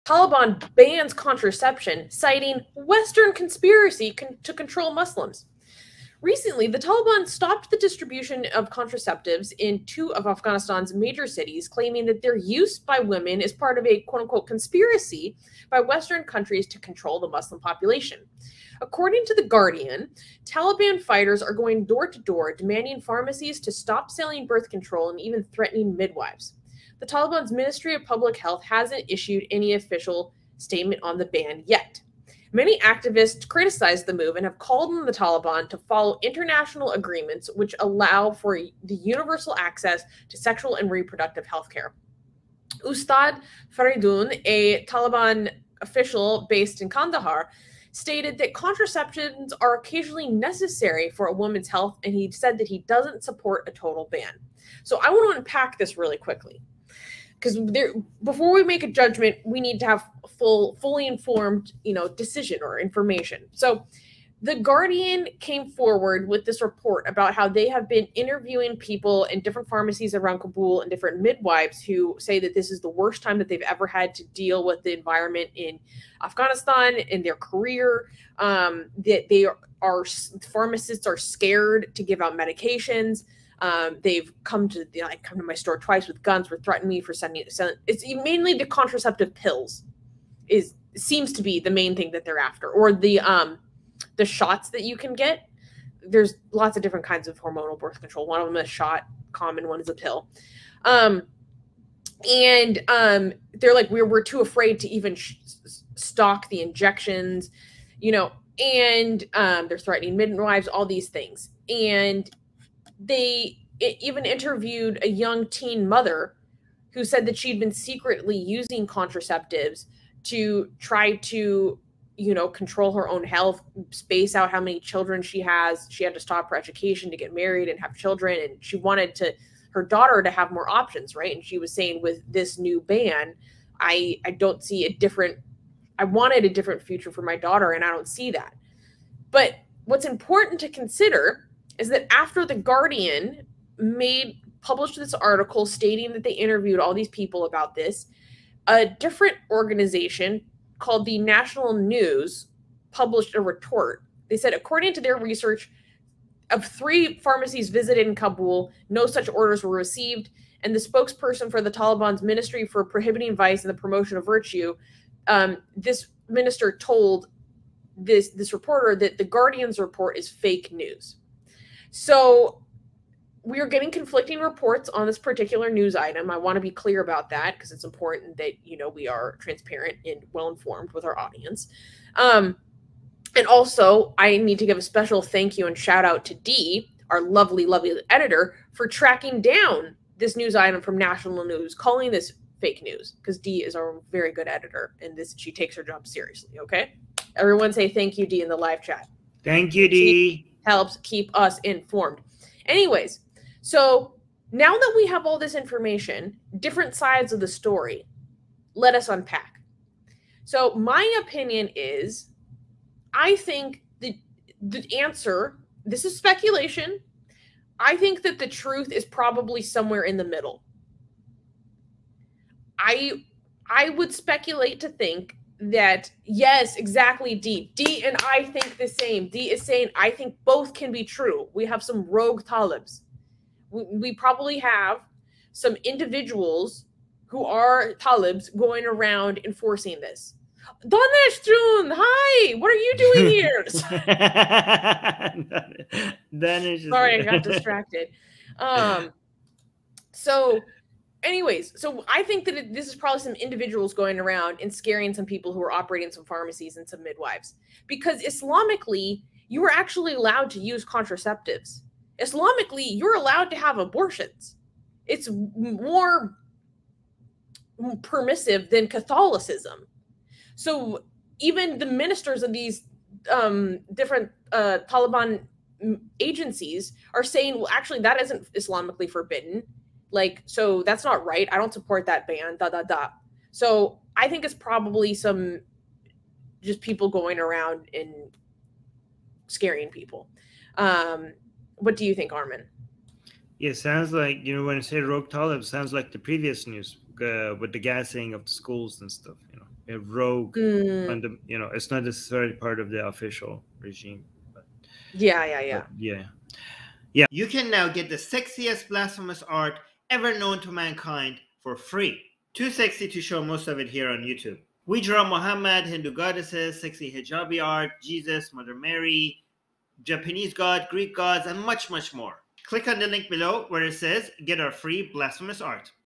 Taliban Bans Contraception, Citing "Western Conspiracy To Control Muslims"
Recently, the Taliban stopped the distribution of contraceptives in two of Afghanistan’s major cities, claiming that their use by women is part of a “conspiracy” by Western countries to control the Muslim population. According to the Guardian, Taliban fighters are going door to door, demanding pharmacies to stop selling birth control and even threatening midwives. The Taliban’s Ministry of Public Health hasn’t issued any official statement on the ban yet. Many activists criticized the move and have called on the Taliban to follow international agreements which allow universal access to sexual and reproductive health care. Ustad Faridoon, a Taliban official based in Kandahar, stated that contraceptives are occasionally necessary for a mother’s health, and he said he doesn’t support a total ban.

Source:
Location: Afghanistan